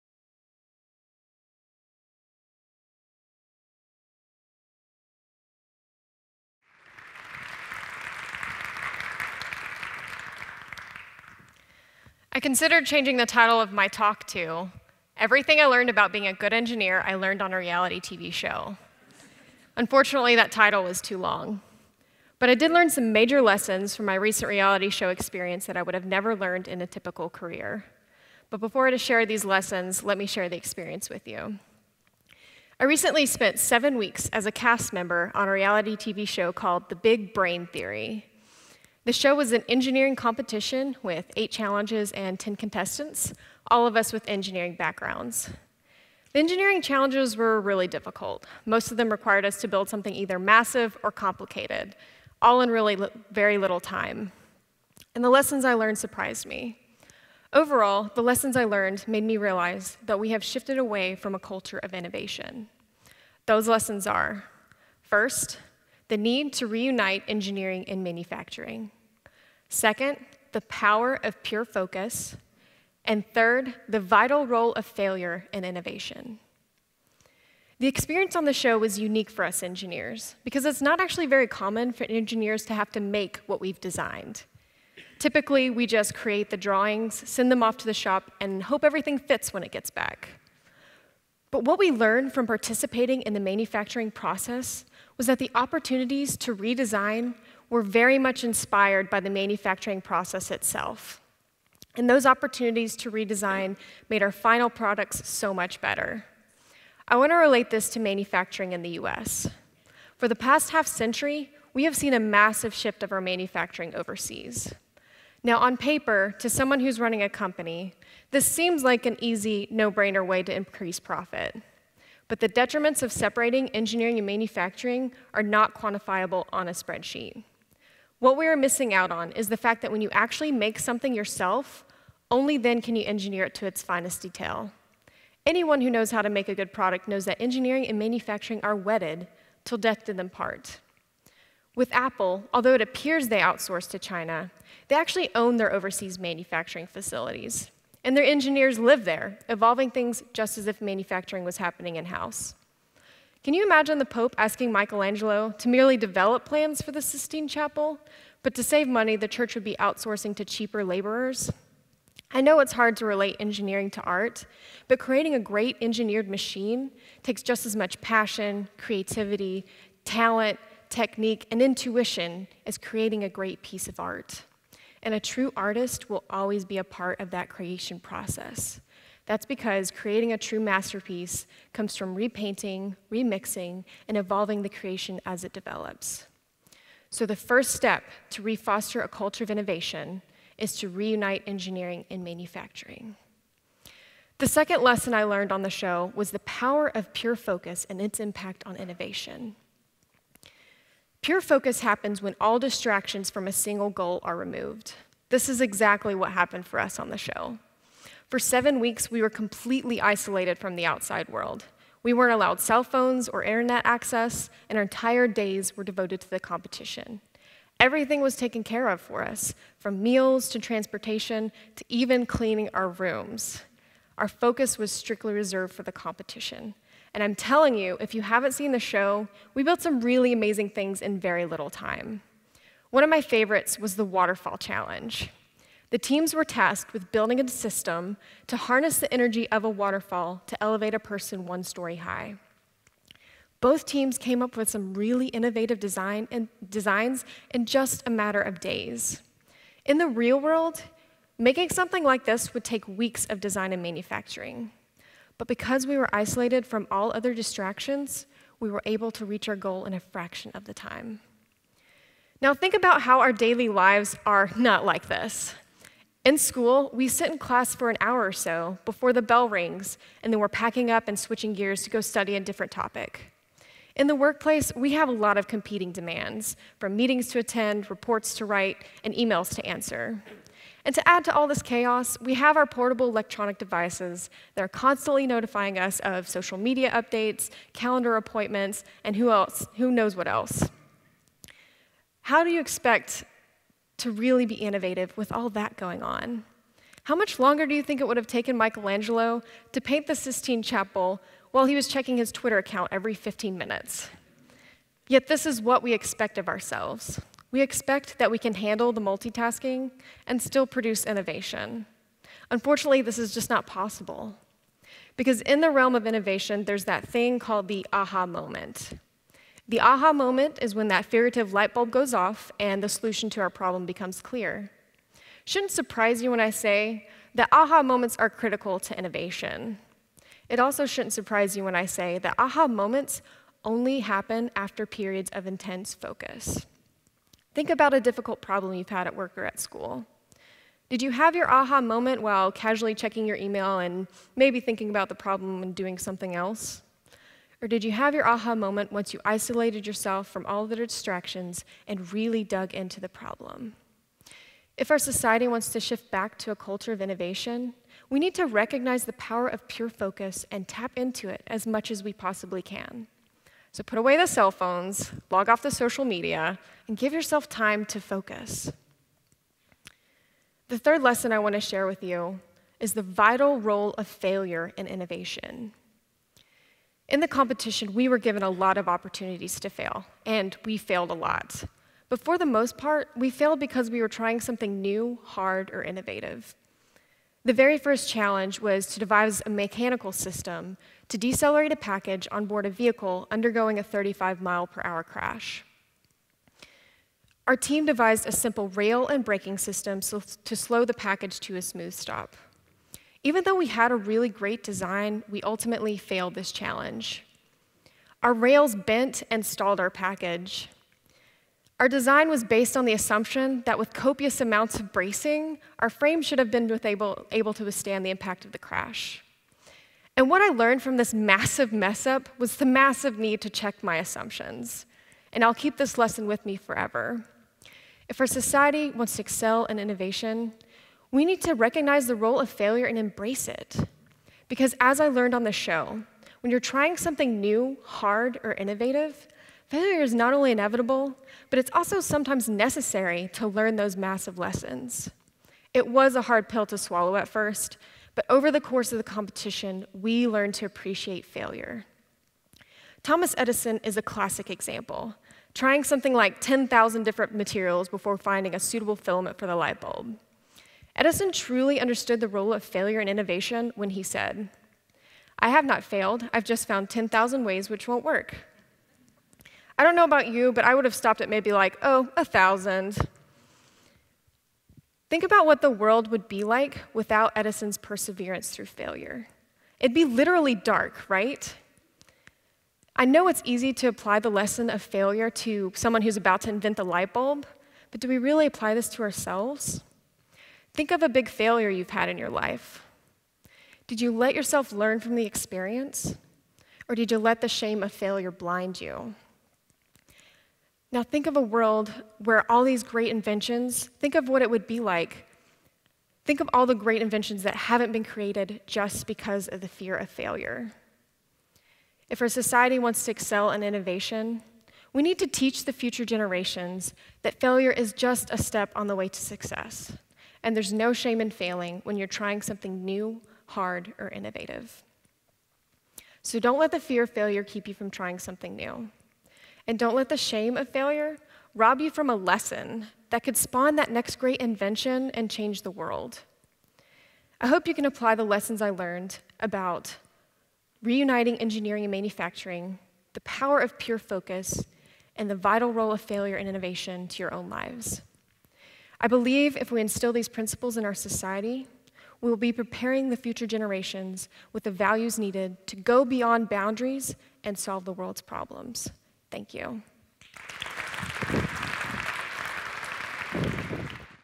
Re-fostering innovation in America: Amy Elliott at TEDxVirginiaTech
Amy Elliott is a PhD Candidate of Mechanical Engineering studying 3D printing. Amy led the design and fabrication of the DreamVendor -- the world's first 3D printing vending machine. Her PhD research deals with creating physical unclonable security features for 3D printed objects. Amy was a finalist on the first season of The Discovery Channel show The Big Brain Theory: Pure Genius, which aired May through June of 2013.  Her talk "Re-fostering Innovation in America" will include some of the insights Amy gained about her own field while being on a reality TV show.